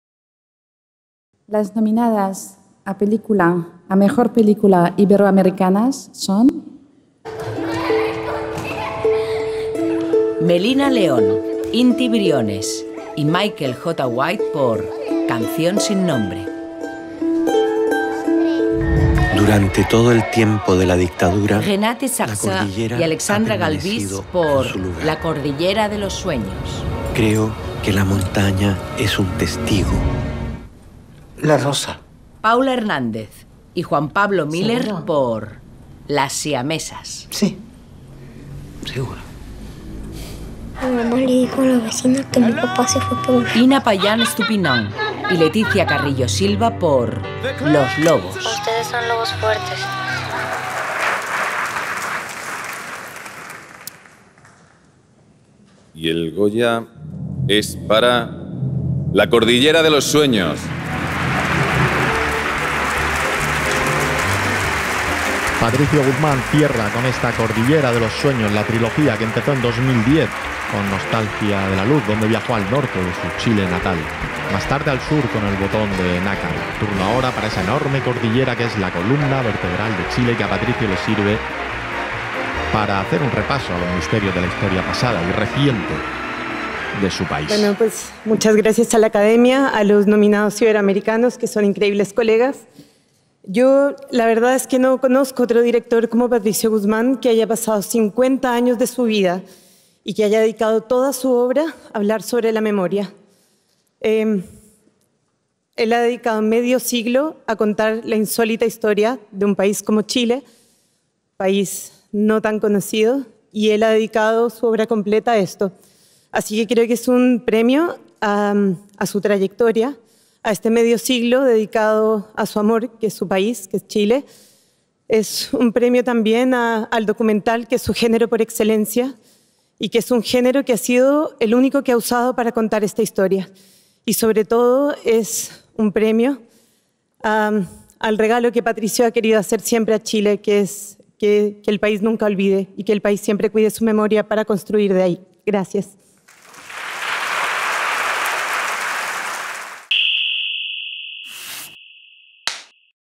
La cordillera de los sueños gana el Goya 2022 a Mejor Película Iberoamericana
La Cordillera de los Sueños (Chile), de Patricio Guzmán, ha sido galardonada con el Goya a Mejor Película Iberoamericana. La productora Alexandra Galvis recibió el premio de manos de Álvaro Morte, Valerie Delpierre y Oscar Jaenada.

La gala de los 36 Premios Goya se celebró el 12 de febrero en el Palau de Les Arts de Valencia.